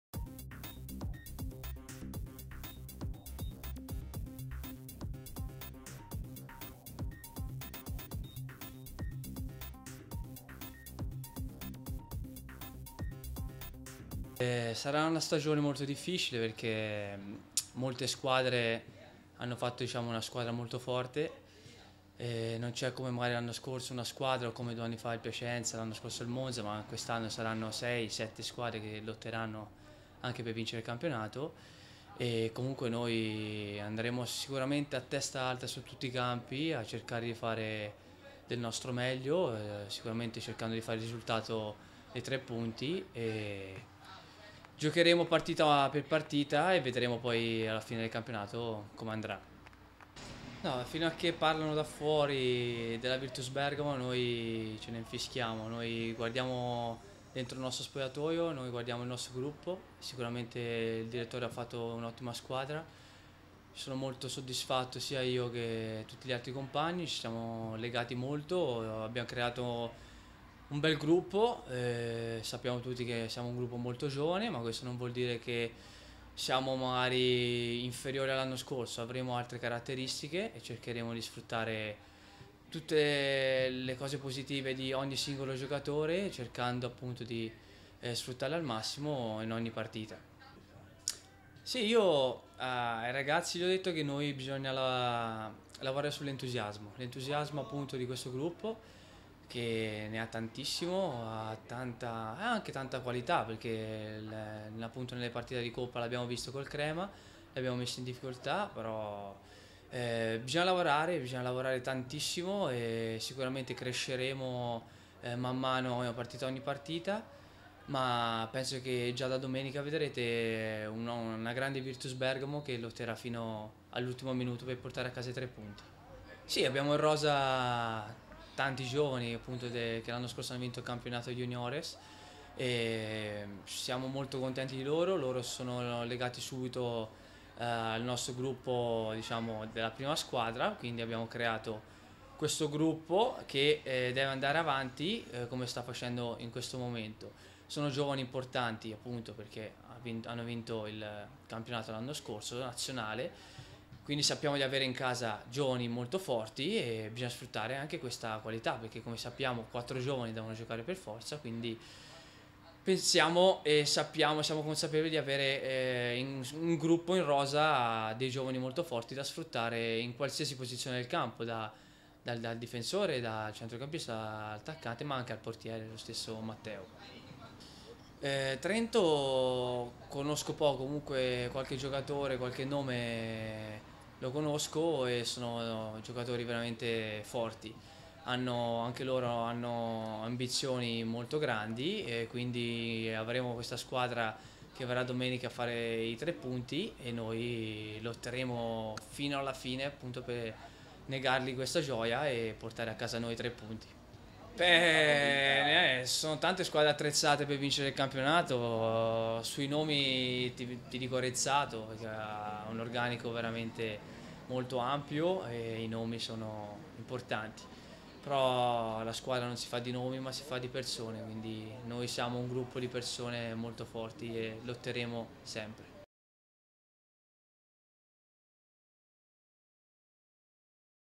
Capitan Alessio Germani ai microfoni di Virtus Bergamo 1909 Channel per il via della stagione
Le impressioni di capitan Germani prima dell'esordio in campionato della stagione 2017/2018